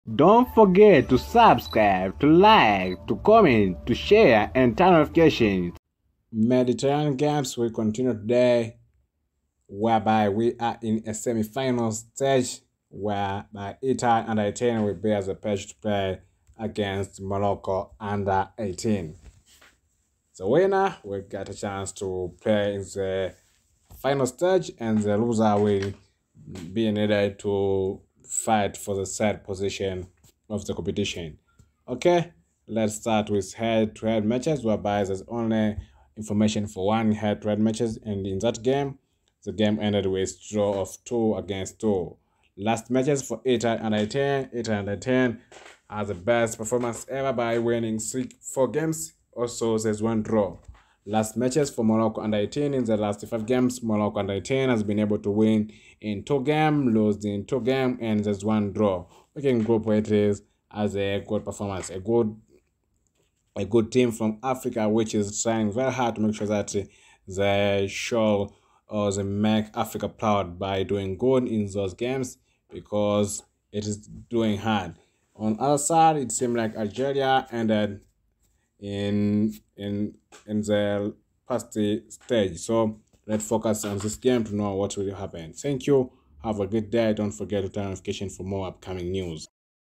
Italy vs Morocco, Mediterranean Games 2022, Semi-Finals / LIVE Match Today.
Italy u18 vs Morocco u18
Mediterranean Games